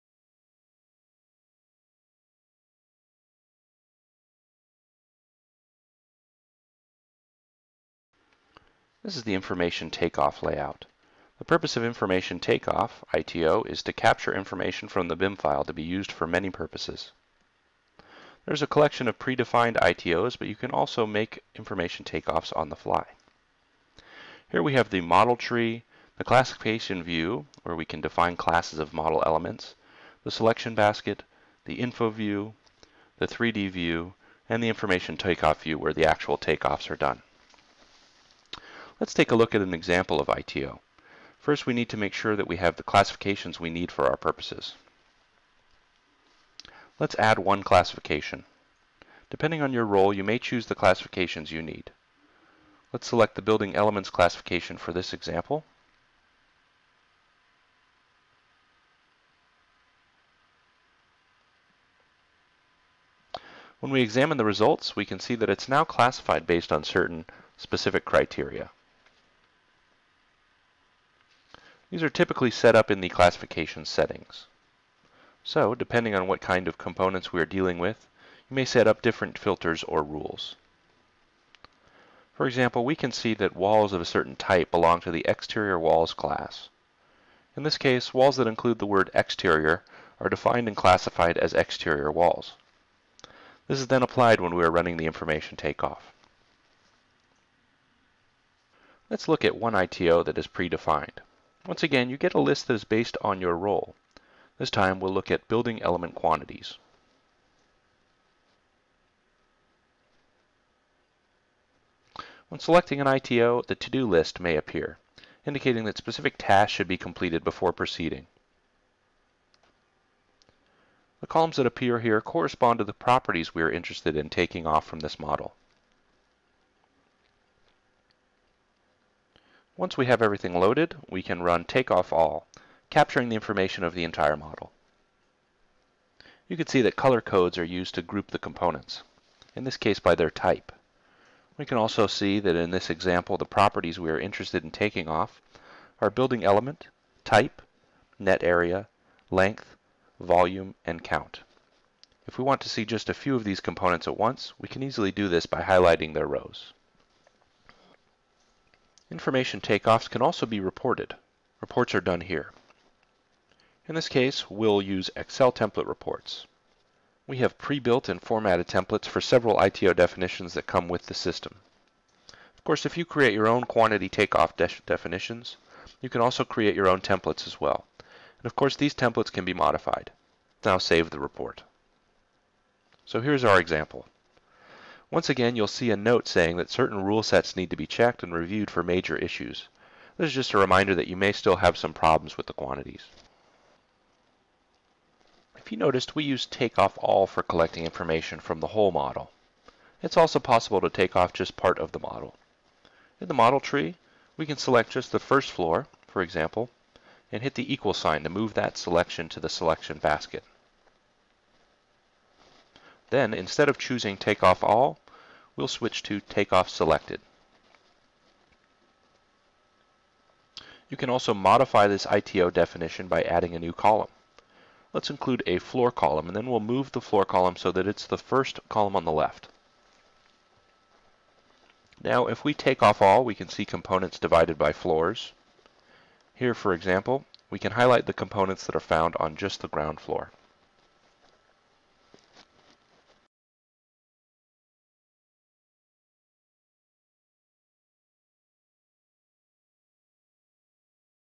Solibri Model Checker Information Takeoff Layout
This video tutorial explains Information Takeoff Layout of Solibri Model Checker v8.